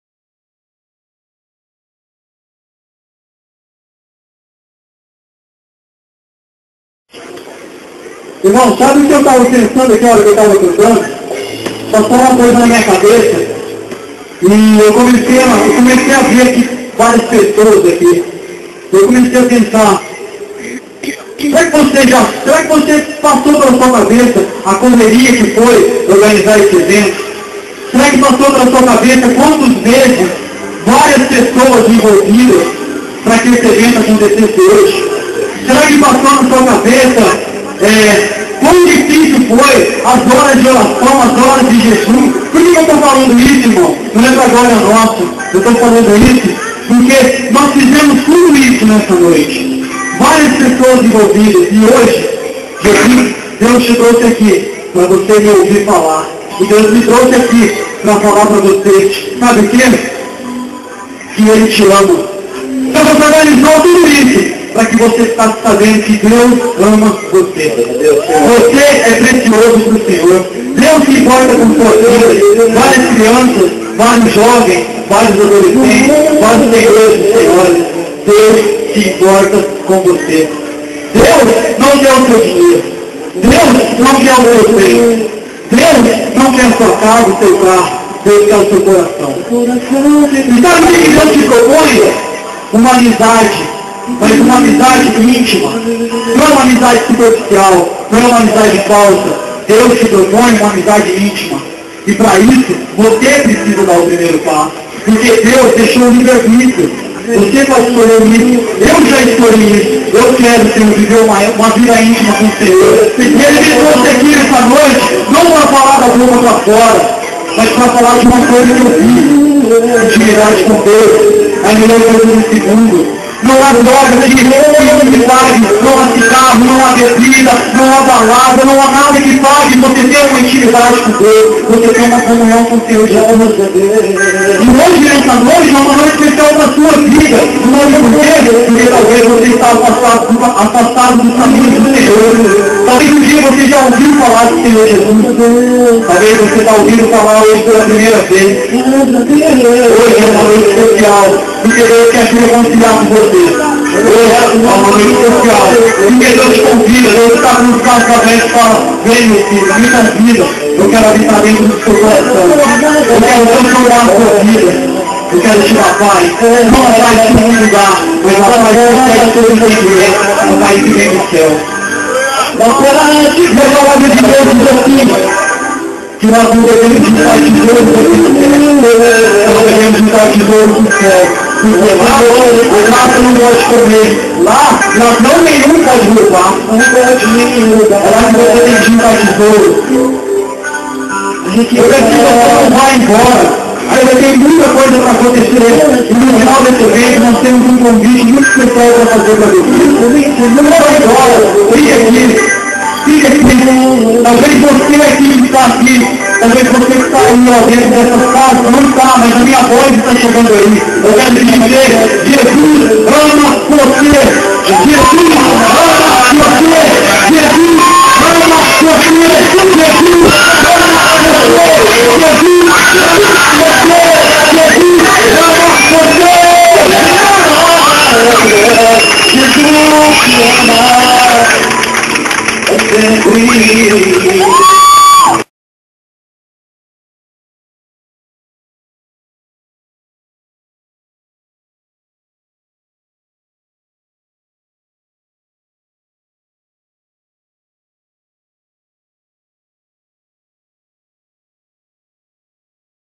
d'Cristo - Ministração (Ao Vivo)
dCristo ministrando a palavra na 4ª Cantata Olhos do Pai.. Filmagem de celular!